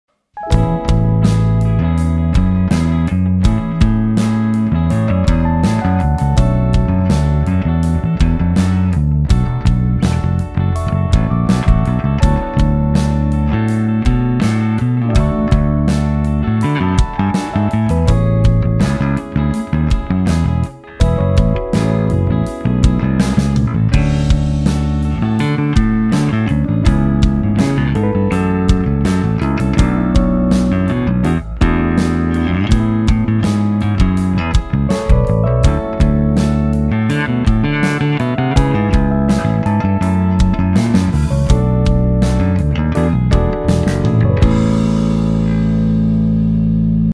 James Jamerson - bass style
Bass line on James Jamerson style
Original bass line by Mastering the Masters - Charles Berthoud

▶︎ Gear
Ibanez SR 4500E DE
Fractal AXE FXII XL+